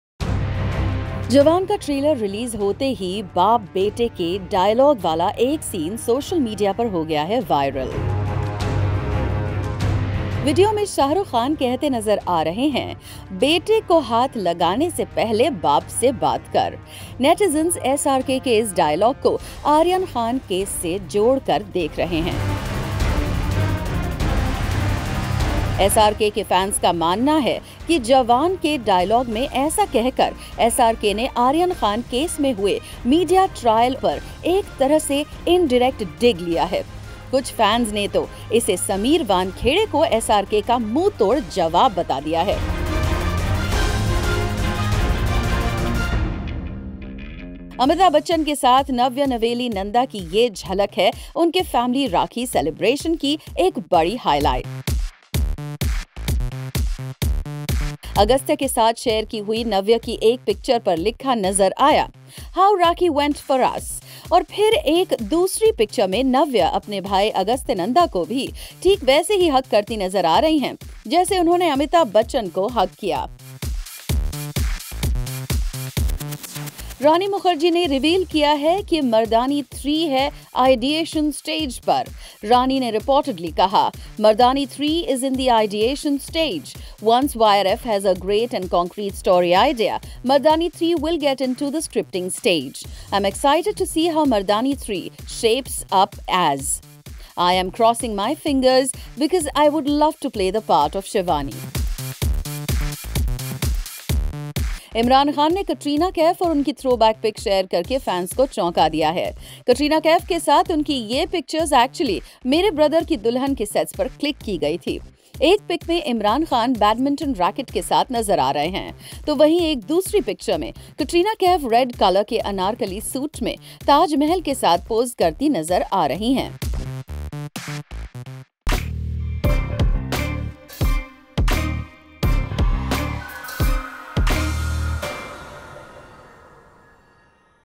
SRK's Jawan dialogue has a connection with son Aryan? | Rani Mukerji is back with Mardaani 3?
Bollywood news: Netizens reacted to the dialogue "Bete ko haath lagane se pahle, baap se baat kar" as soon as the trailer for Shah Rukh Khan's Jawan was trailer was released. Navya Naveli Nanda, Amitabh Bachchan's granddaughter, uploaded photos from the Raksha Bandhan 2023 celebration with her brother Agastya Nanda. Big B was also in one of the photos!